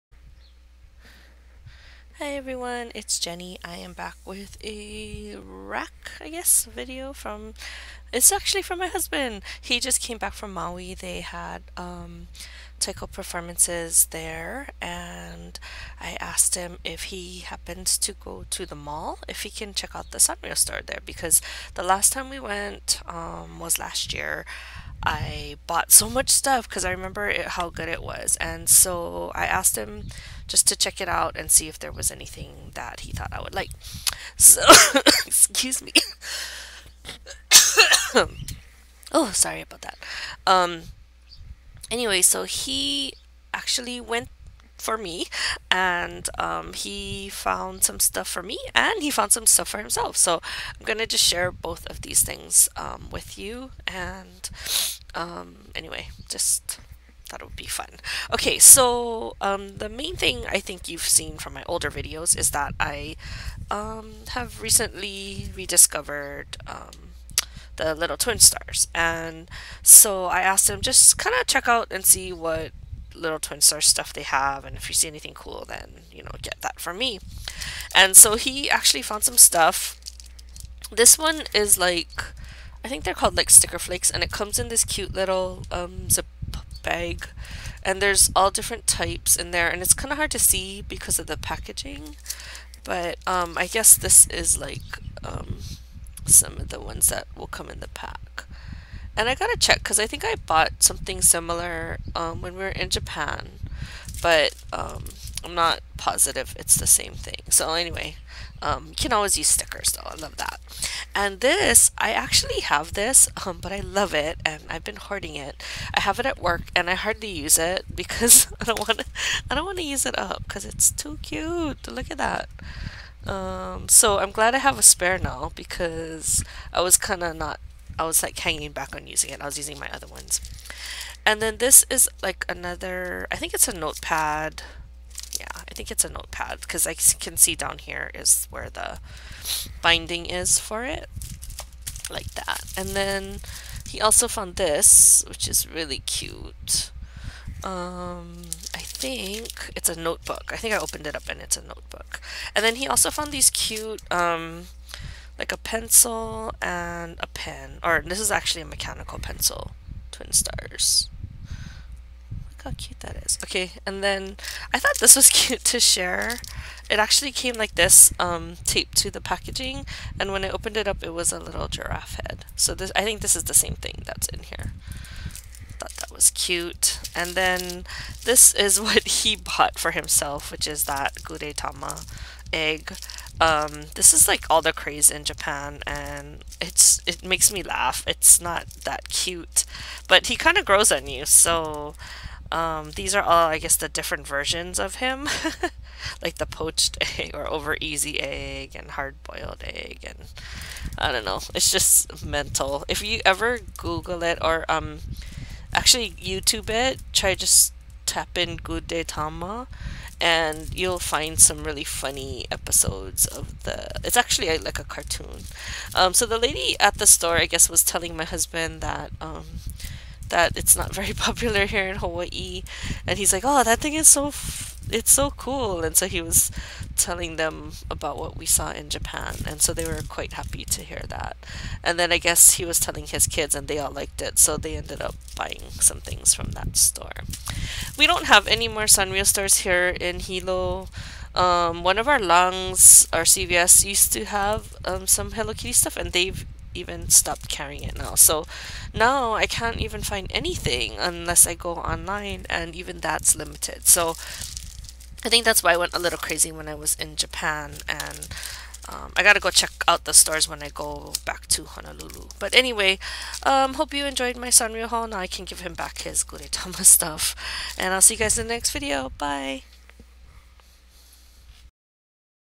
Sanrio RAK!
Hubby came back from Maui and brought me some Little Twinstar goodness!